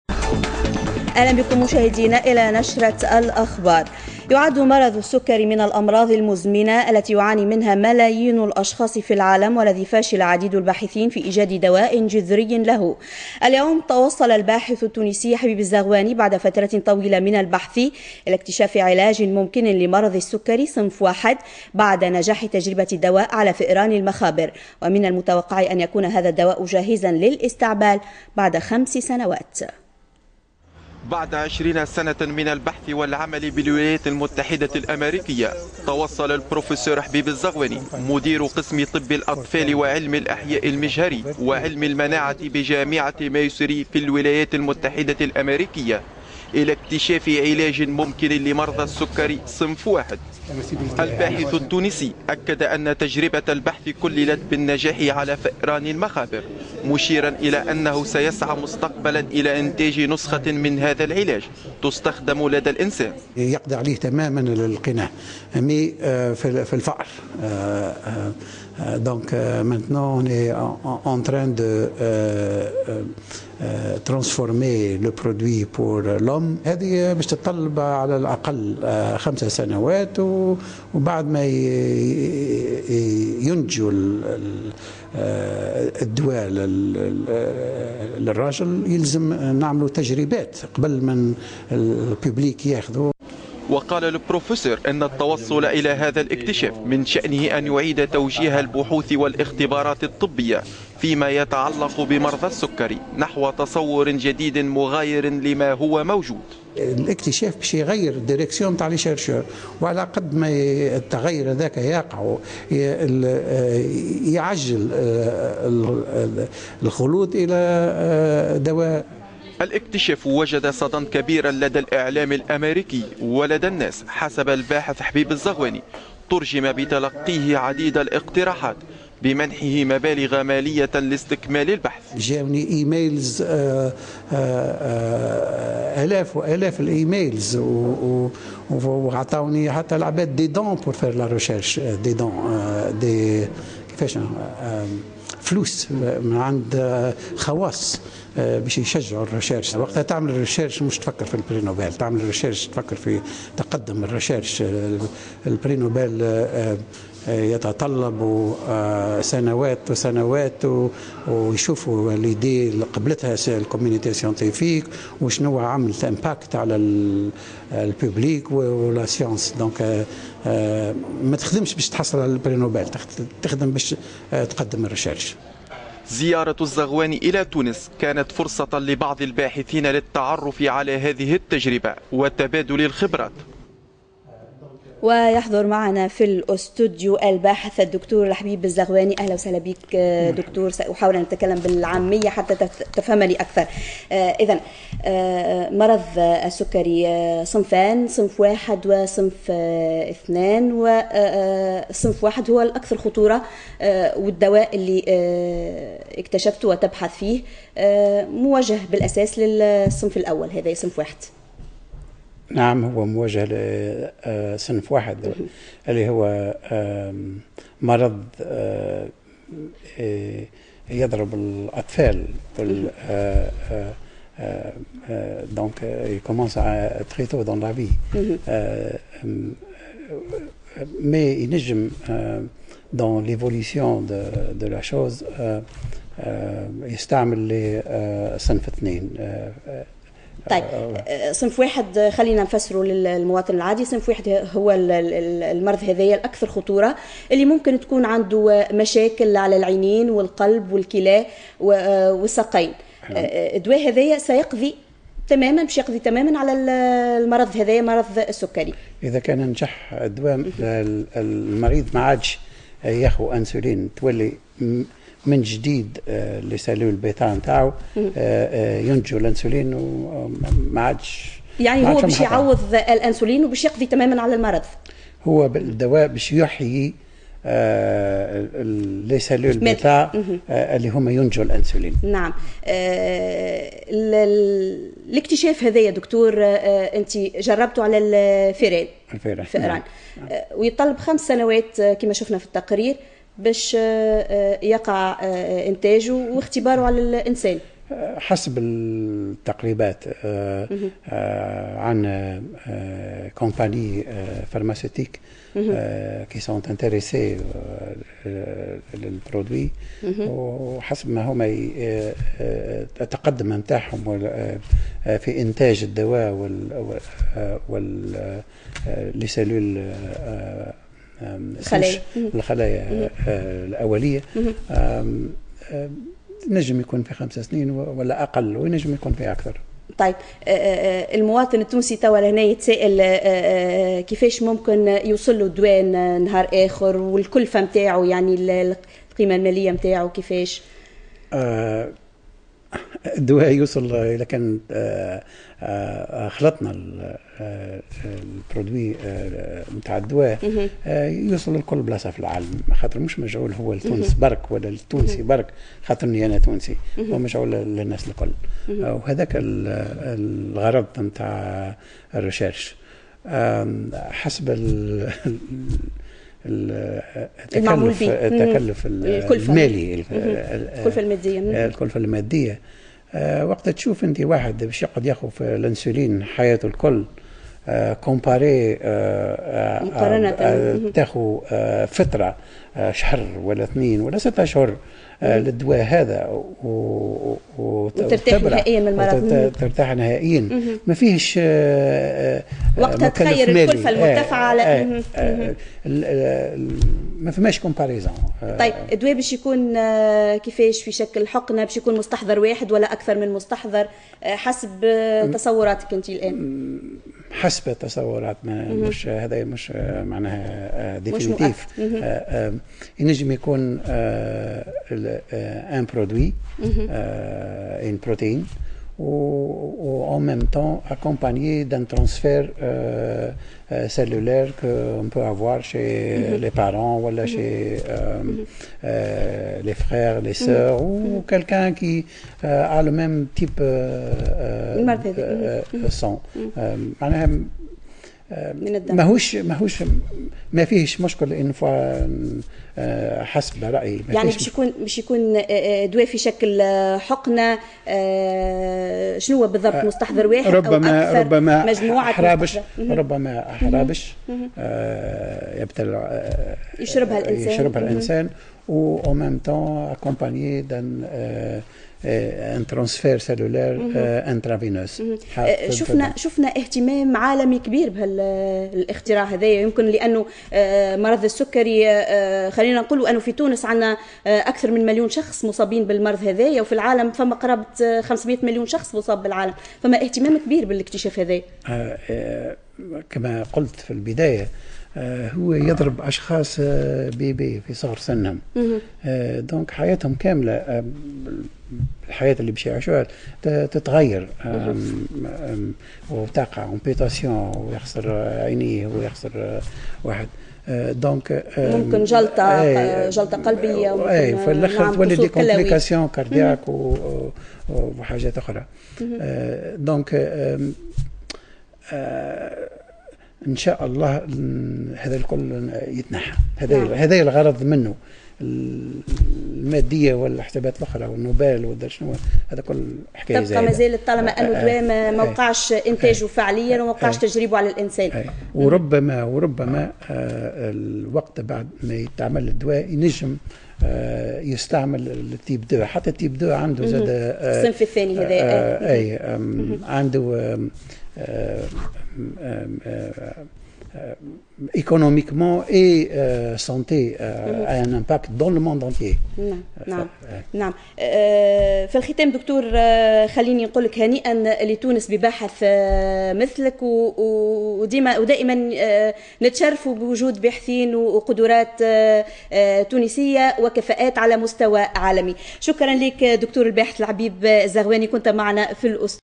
طبيب تونسي مقيم في أمريكا يكتشف علاجا ممكن لمرضى السكري 13 02 2014
يعد مرض السكري من أمراض المزمنة التي يعاني منها ملايين الأشخاص في العالم و الذي فشل عديد الباحثين في إيجاد دواء جذري له، اليوم توصل الباحث التونسي الحبيب الزغواني بعد فترة طويلة من البحث لإكتشاف علاج ممكن لمرض السكري صنف واحد بعد نجاح تجربة الدواء على فئران المخابر و من المتوقع أن يكون هذا الدواء جاهزا للإستعمال بعد خمس سنوات